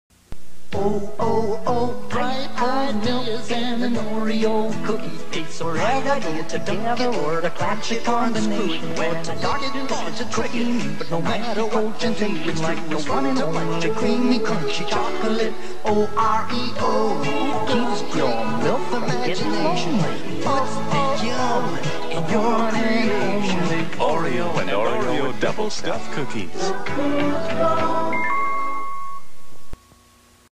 Oreo Commercial 1983 and 1985? Comparison
I know the left is 1983 but the right? It might be 1985.
These commercials sound so similar so I did a comparison on them.